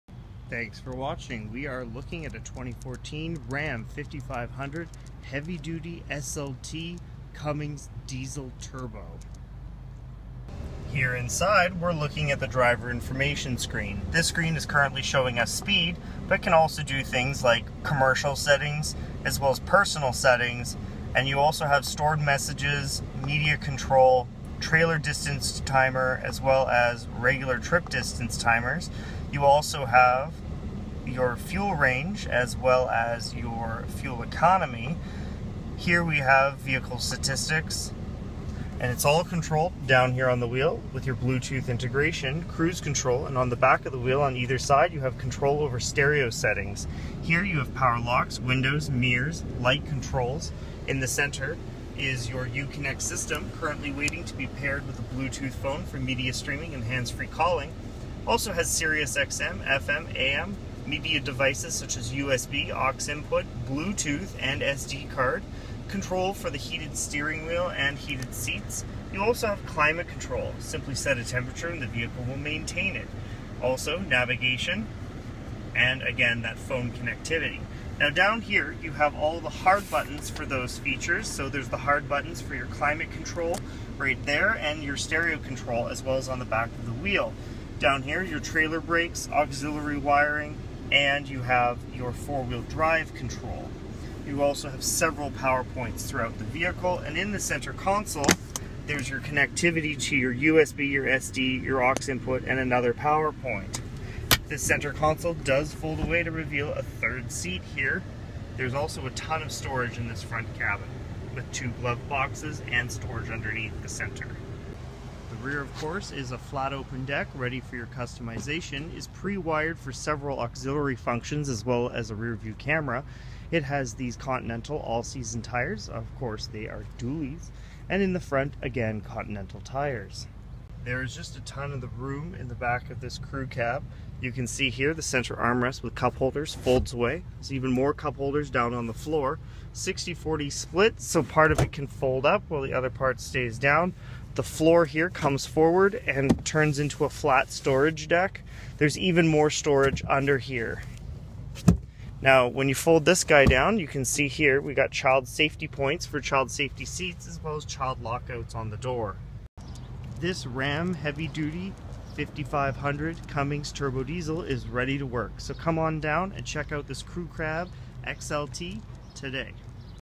2014 Ram 5500 4WD Crew Cab 197" WB 84" CA SLT
This is a  2014 Ram 5500 4WD Crew Cab 197&quot; WB 84&quot; CA SLT with 6-Speed A/T transmission White[Bright White Clearcoat] color and Gray interior color.

Great West Chrysler
17817 Stony Plain Road
Edmonton, Alberta
T5S 1B4